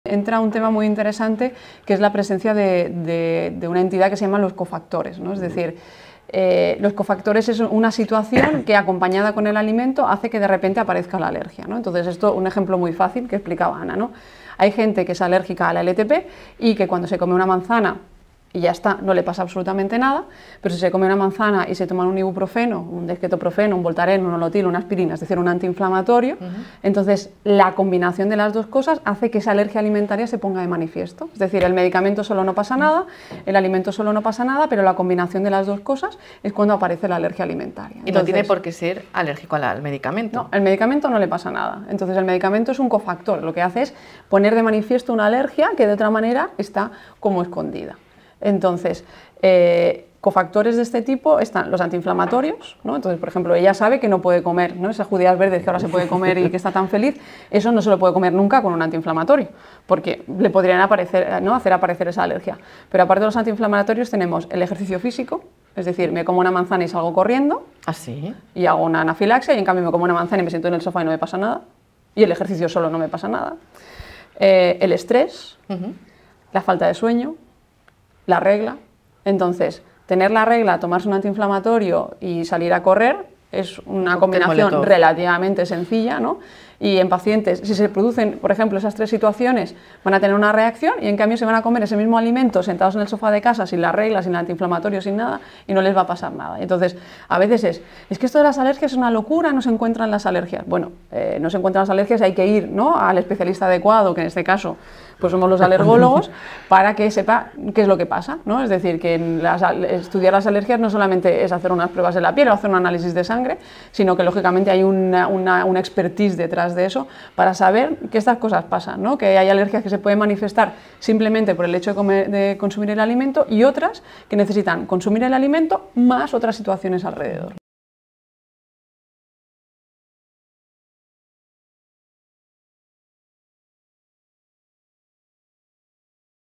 ¿Puede ser que un mismo alimento a veces de alergia y otras veces no? | PortalCLÍNIC
La Dra. Rosa Muñoz y el Dr. David Loli, alergólogos del hospital Clínic Barcelona, responden diferentes dudas sobre las alergias alimentarias.